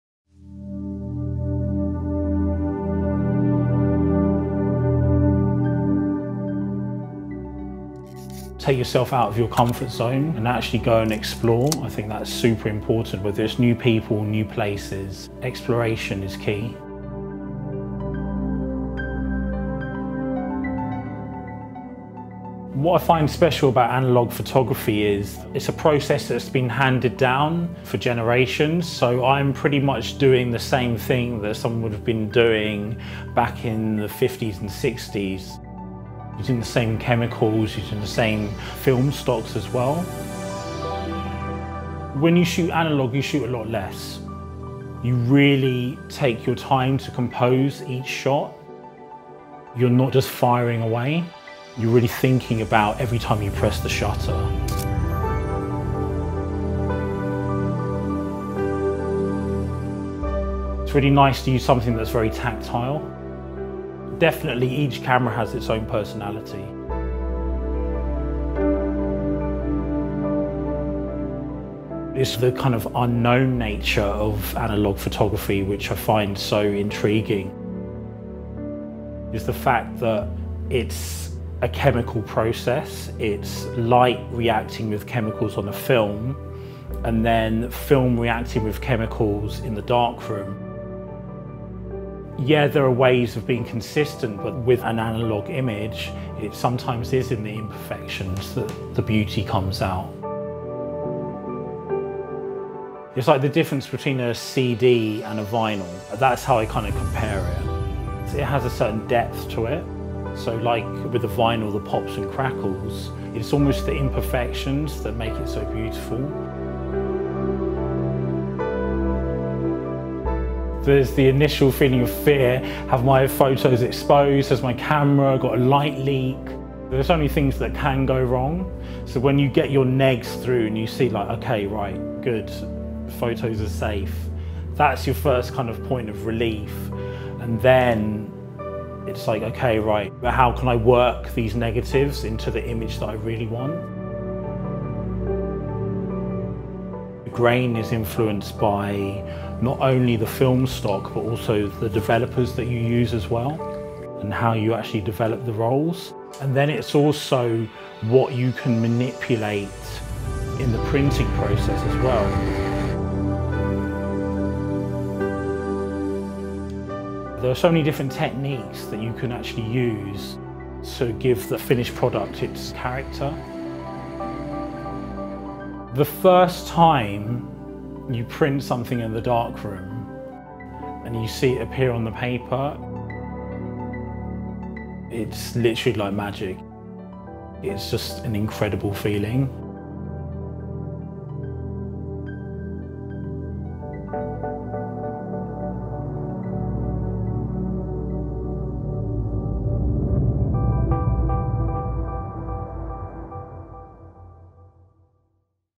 The Art of: Analogue Architecture Photography with Ollie Trenchard
As part of our ongoing film series exploring modern craft, we spoke to photographer Ollie Trenchard about what his practice means to him.

As the digital era reigns on, the recent resurgence of tactile activities and traditional practices have become more valued in our everyday lives. Whilst our screens are congested with a constant stream of content, many of us are now tempted by a slower approach. In fact, it's often the union between artisanal craft and more contemporary skills that allow us to capture creativity so successfully.

As part of our ongoing film series exploring modern craft, we spoke to photographer Ollie Trenchard (@ollietrenchard) about what his practice means to him. 'The Art of Analogue Photography' takes us through Ollie's creative process, from the initial exploration of a space to the chemical development of the print.

"When you shoot analogue, you shoot a lot less. You really take your time to compose each shot, you're not just firing away, you're really thinking about every time you press the shutter."